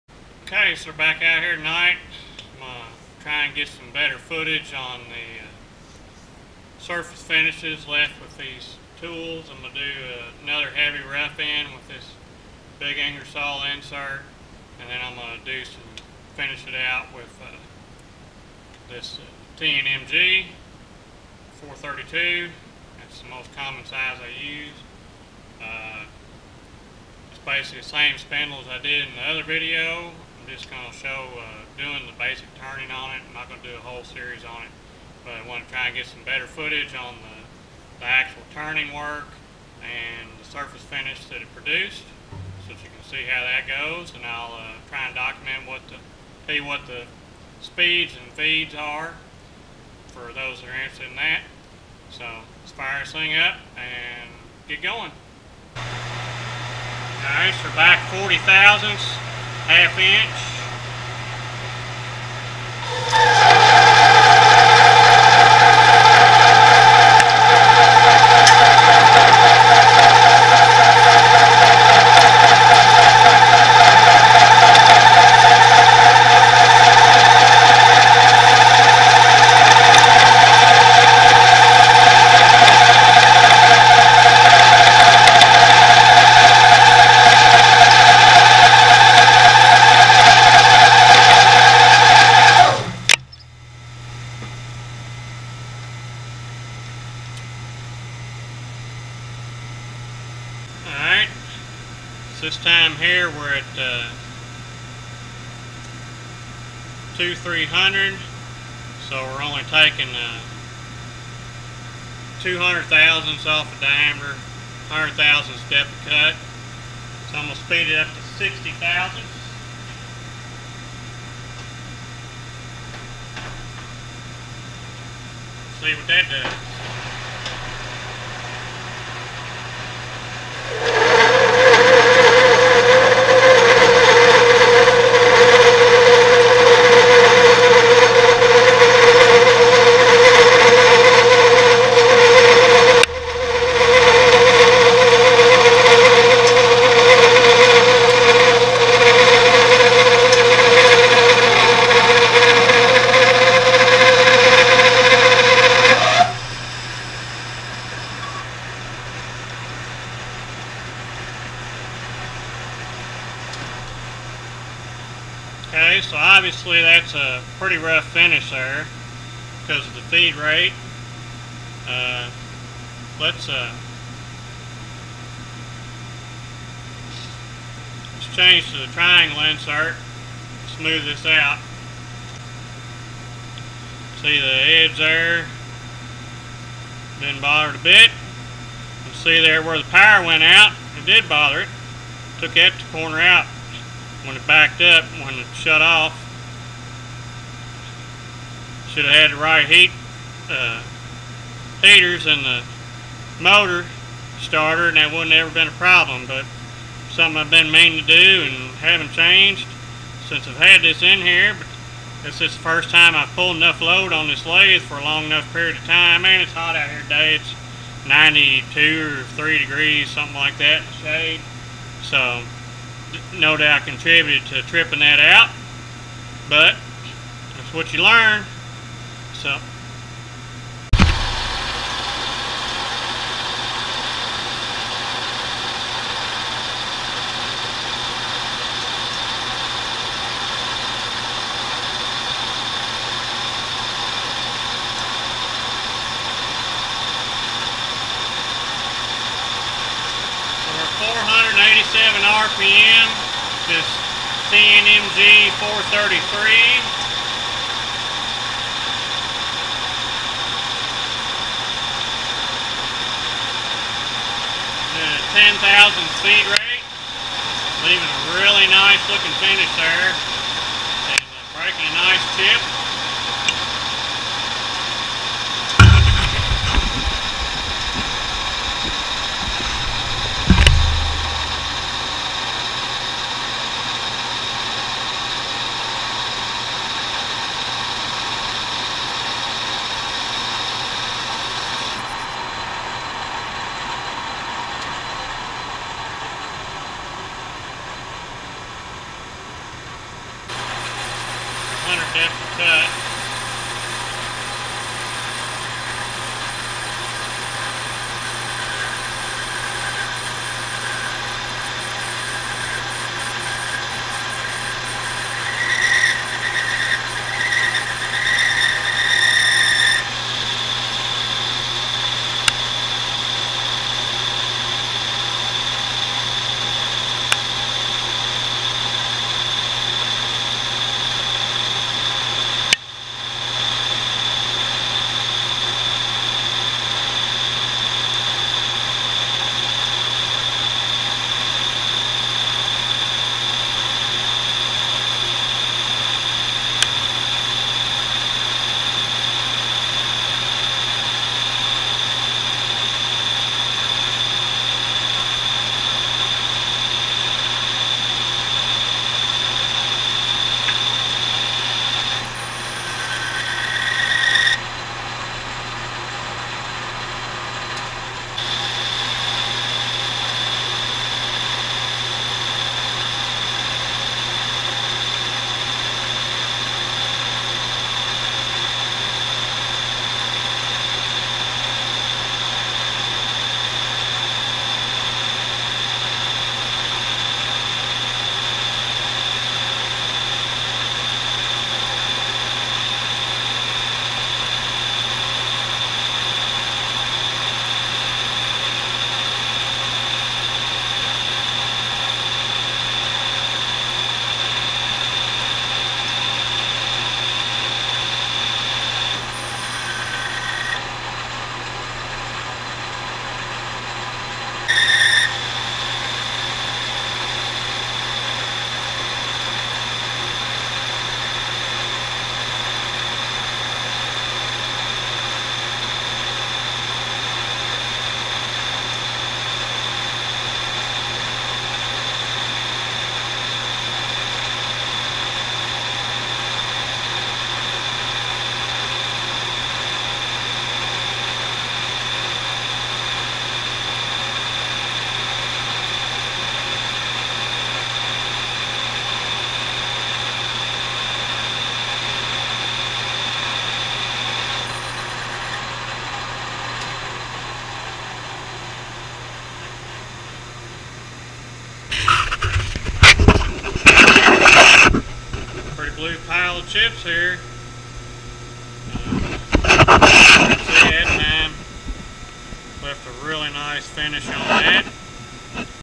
Chip Breaking
Showing effects of a couple inserts with various speeds, Depth of Cut, and feed rates on surface finish on fatigueproof steel. Basically this is just a results video of what I found to work and what I didn't. It is mostly all turning the entire time so sit back and watch the chips fall!